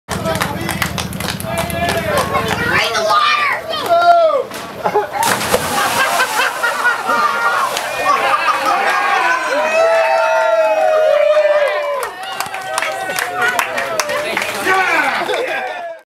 Super Dan Takes The Plunge
Video of Danny A. "Hurricane Halmo" riding his e-bike into the Puce River in Lakeshore, Ontario while completing his cross-Canada Ride-Along of Pos-Abilities on Tuesday September 11, 2012.   Danny Halmo's trip across Canada has raised awareness for people with disabilities and those who drive electric vehicles.  (Video By: Jason Kryk)